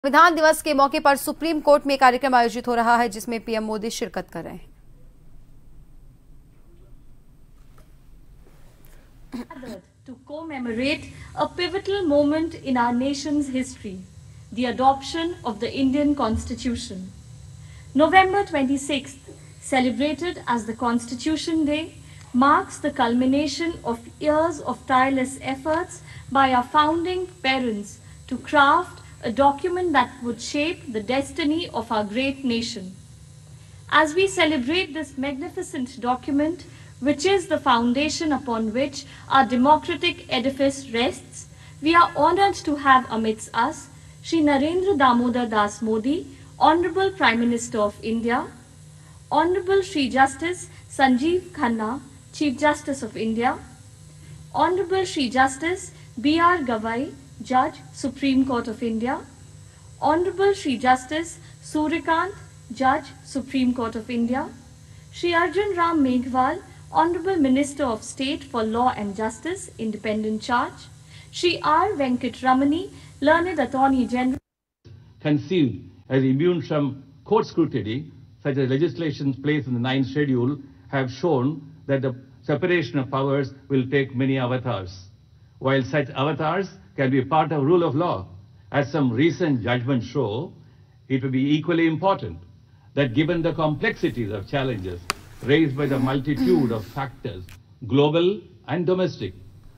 Live | सुप्रीम कोर्ट में संविधान दिवस पर समारोह, समारोह में प्रधानमंत्री नरेंद्र मोदी मौजूद | JAN TV
Watch JAN TV on :
Tata Play DTH : 1185
Airtel DTH: 355
JIO Fiber: 1384

News Credit-MKP